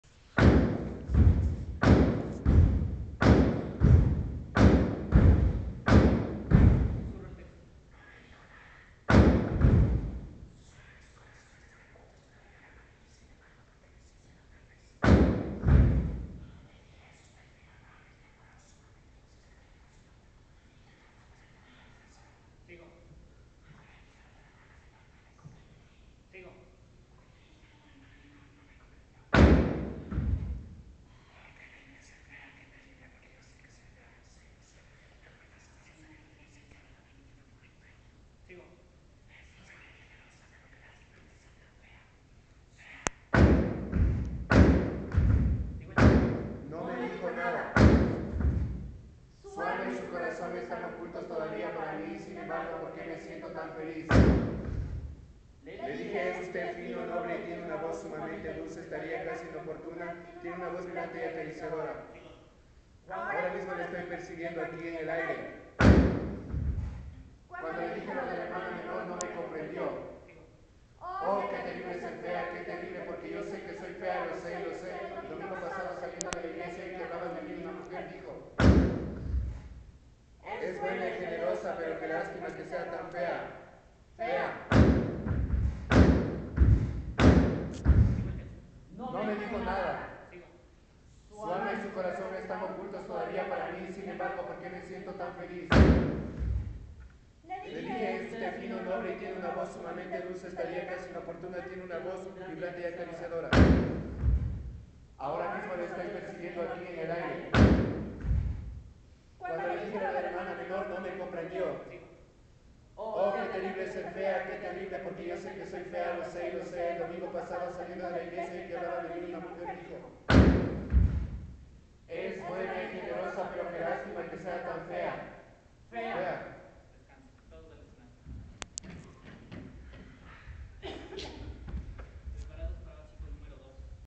Suzuki Method 3
Básico número 3

Compañía Estudio de Actores
Vania, una Comedia Negra de Anton Chejov
Suzuki Method
18 de noviembre 2012
El Teatro - Quito - Ecuador